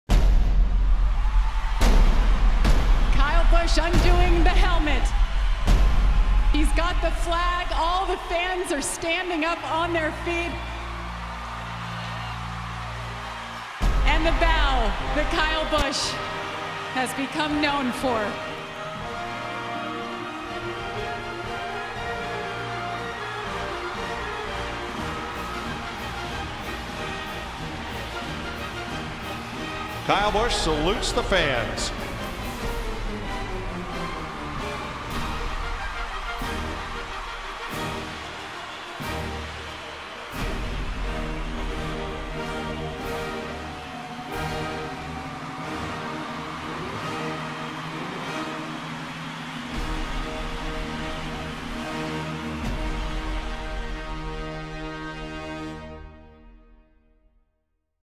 Kyle Busch takes a bow ... and a bow ... and a bow
See a selection of Kyle Busch's trademark celebration -- the bow -- that has been performed 200 times in NASCAR.

Play every weekend!